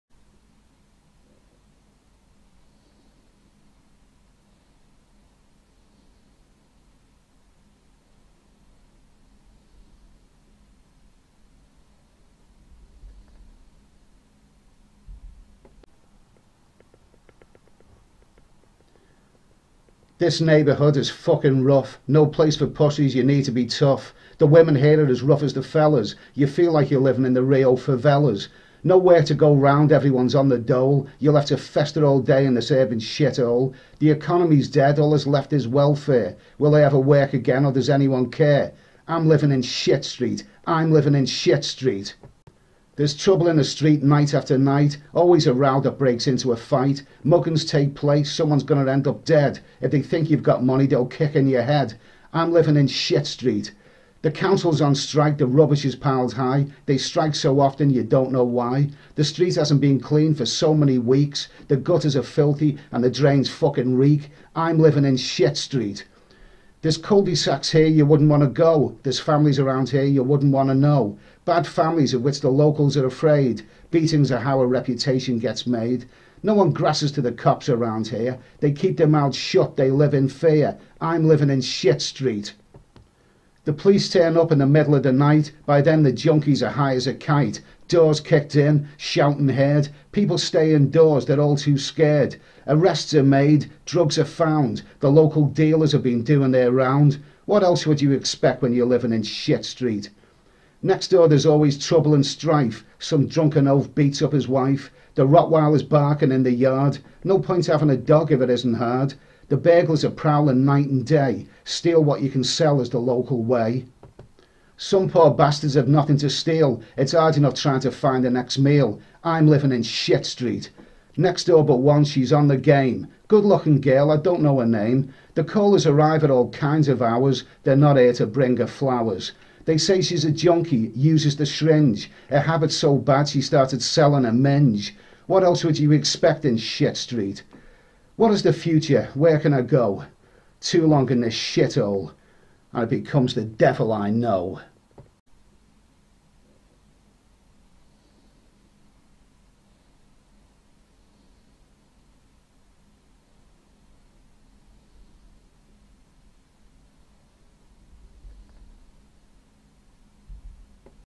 Shit Street Ed Sweeney
Thought provoking poem about deprived areas.  A real street which someone had appropriately renamed.  Crime, drugs and violence are a way of life in this run down neighborhood.